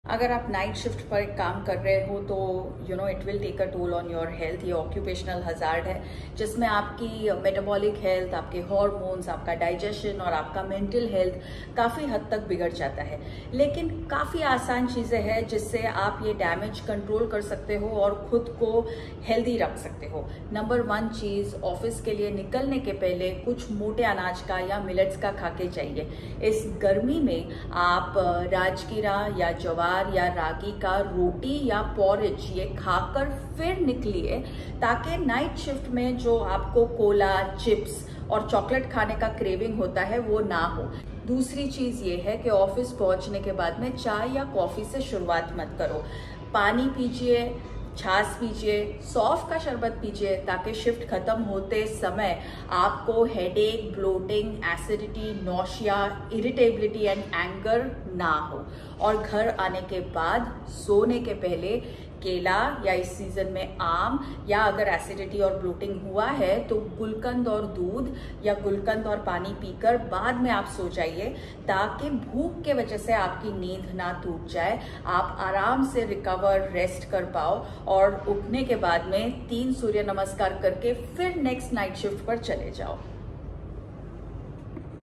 3 food tips for night shifts
3 food tips for night shifts -

1. Before leaving from home, have a millet based meal like ragi porridge, etc.
2. After reaching office have water or buttermilk or a seasonal sherbet.
3. Before sleeping have a banana or gulkand with milk or water.